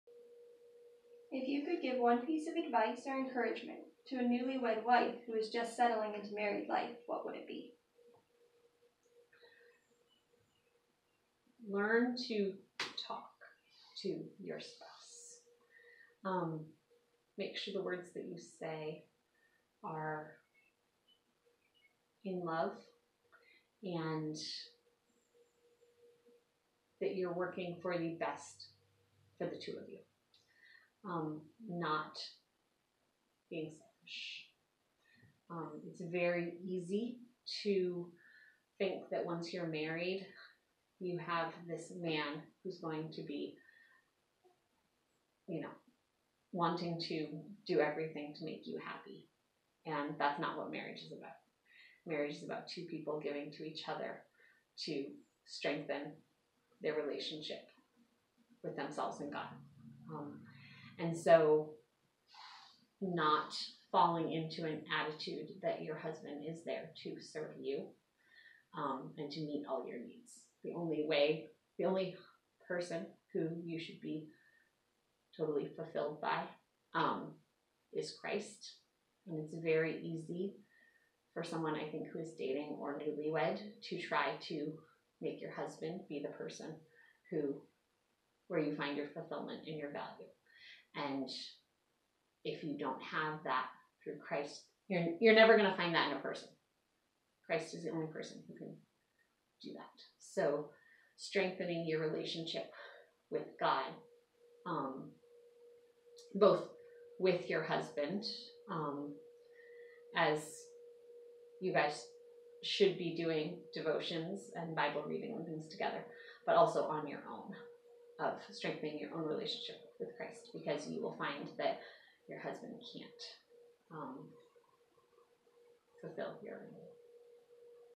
Answering your questions- newlyweds
What is your advice for those recently married? Sara shares her experience and some Biblical wisdom on the topic of newlyweds.